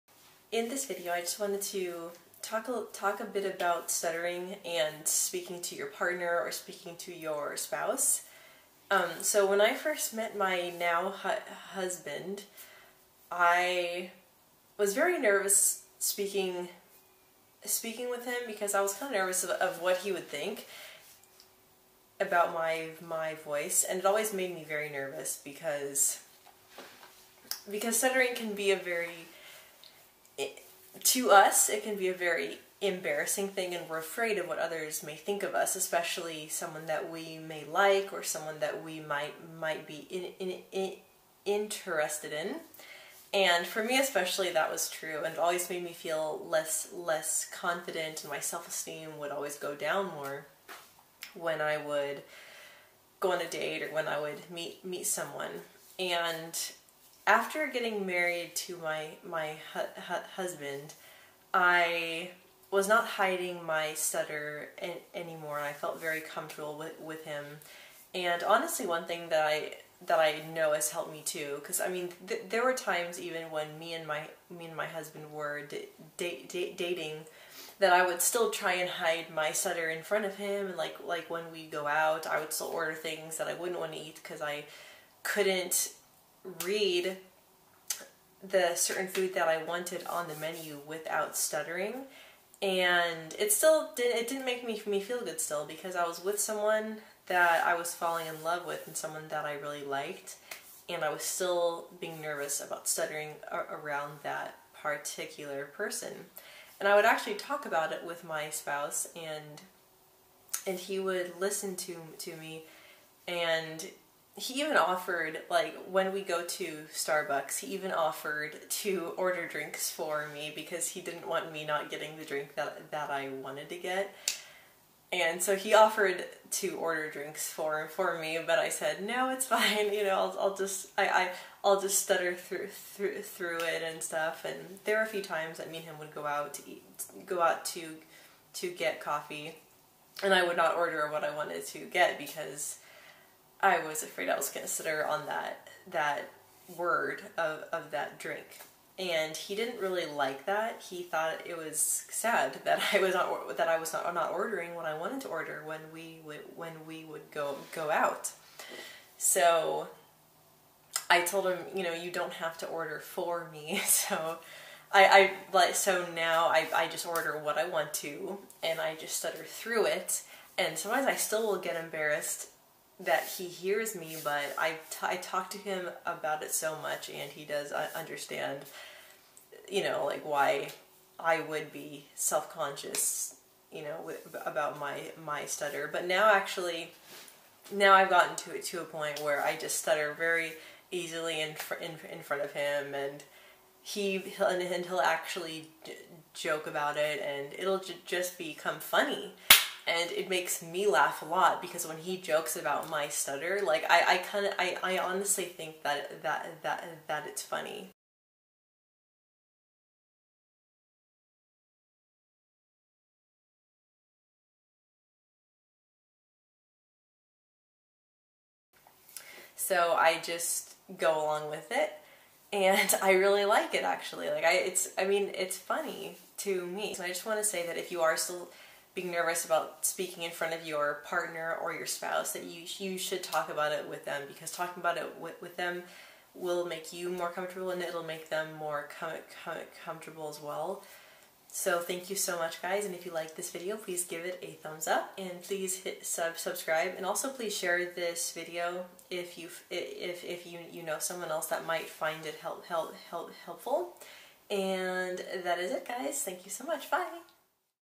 Stuttering vs Spouse/Significant Other
Stuttering and Dating

Hello everyone!

I hope you are all doing well. In this video I talk about stuttering and my husband and why I discuss my stuttering with my husband a lot. I talk about how discussing your stuttering with your spouse/significant other is so important. I hope you like it!